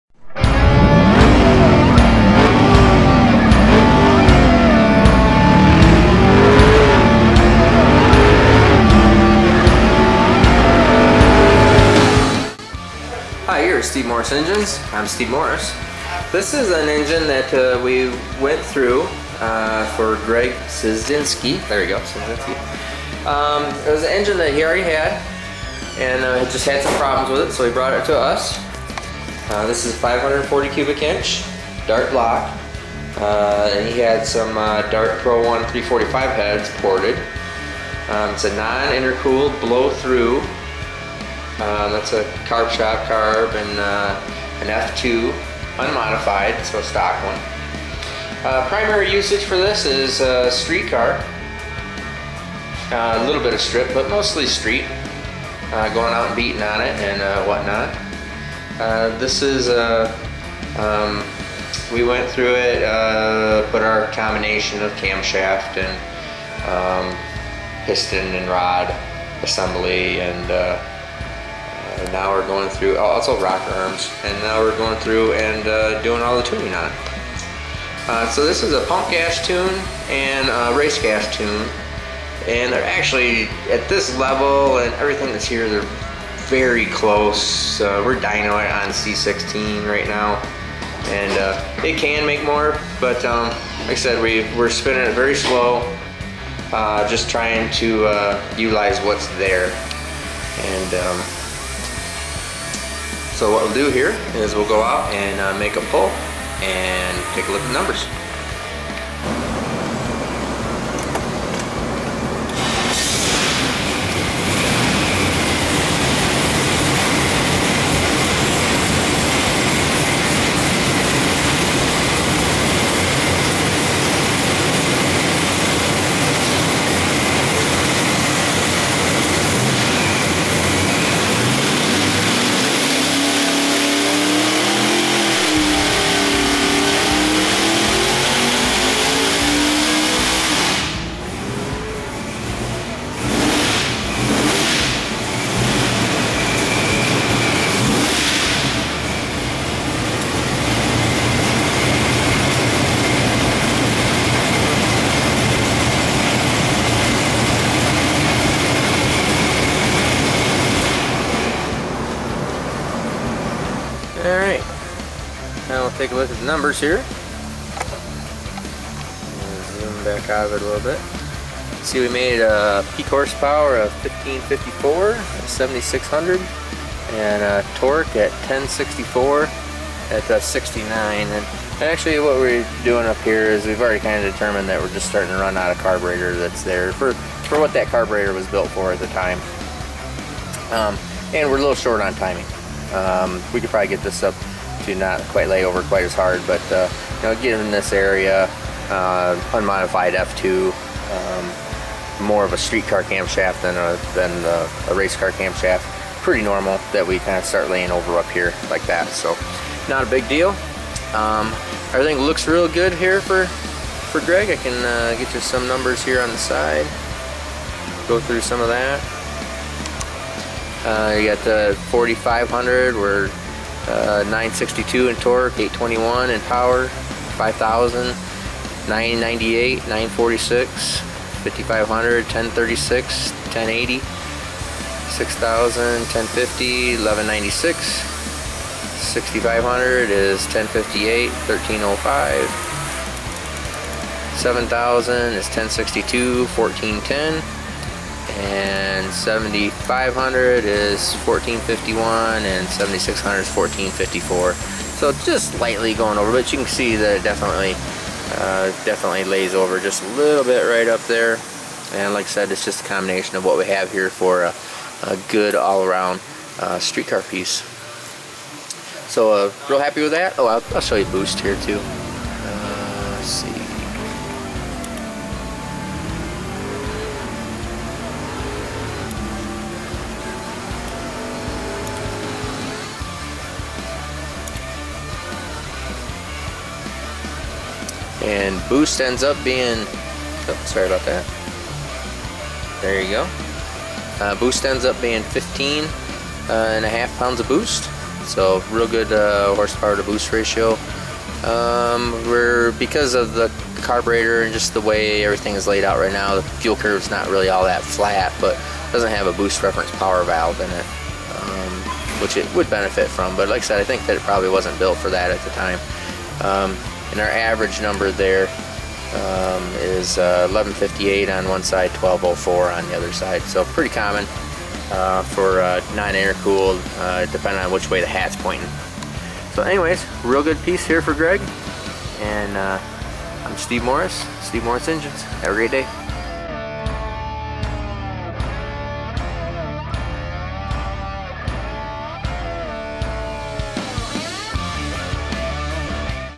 Street strip BBC ProCharged
Pump-gas friendly, non-intercooled BBC engine package from STEVE MORRIS ENGINES.